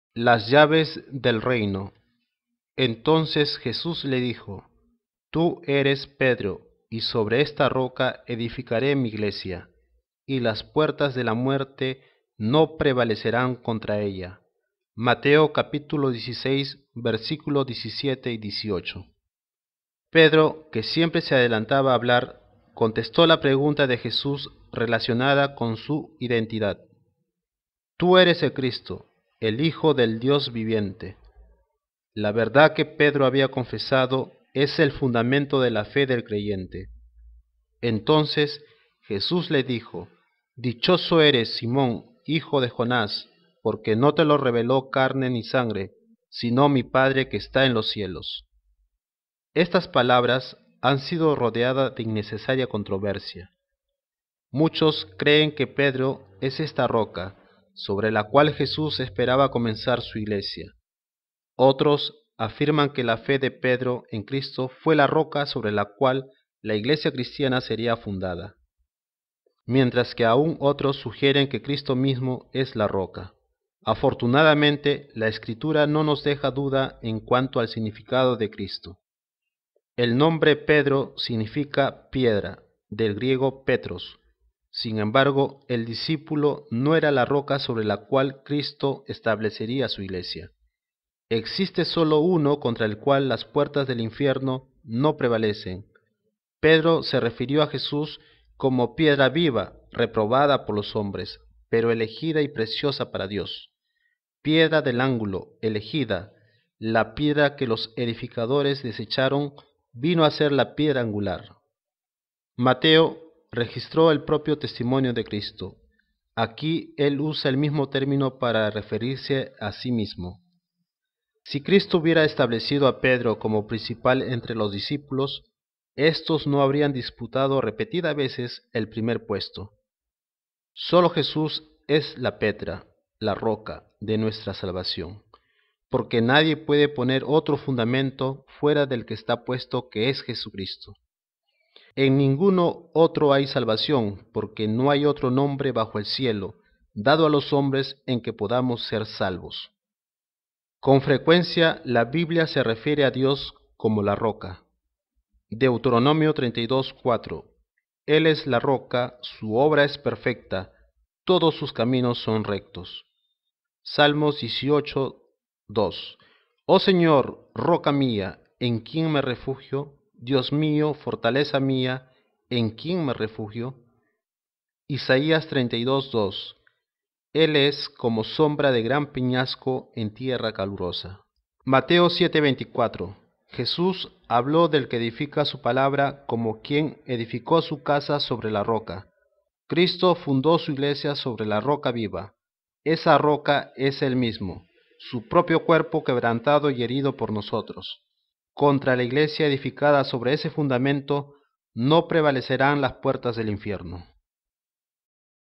Las llaves del reino - Mateo 16:17,18.
La confesión de Pedro.
Mateo 16: 13-20 (Mr. 8.27-30; Lc. 9.18-21)
"Viniendo Jesús a la región de Cesarea de Filipo, preguntó a sus discípulos, diciendo: ¿Quién dicen los hombres que es el Hijo del Hombre? Ellos dijeron: Unos, Juan el Bautista; otros, Elías; y otros, Jeremías, o alguno de los profetas. El les dijo: Y vosotros, ¿quién decís que soy yo? Respondiendo Simón Pedro, dijo: Tú eres el Cristo, el Hijo del Dios viviente.

"Entonces le respondió Jesús: Bienaventurado eres, Simón, hijo de Jonás, porque no te lo reveló carne ni sangre, sino mi Padre que está en los cielos. Y yo también te digo, que tú eres Pedro, y sobre esta roca edificaré mi iglesia; y las puertas del Hades no prevalecerán contra ella. Y a ti te daré las llaves del reino de los cielos; y todo lo que atares en la tierra será atado en los cielos; y todo lo que desatares en la tierra será desatado en los cielos.

"Entonces mandó a sus discípulos que a nadie dijesen que él era Jesús el Cristo".

Reina-Valera 1960 (RVR1960)
Versión Reina-Valera 1960 © Sociedades Bíblicas en América Latina, 1960.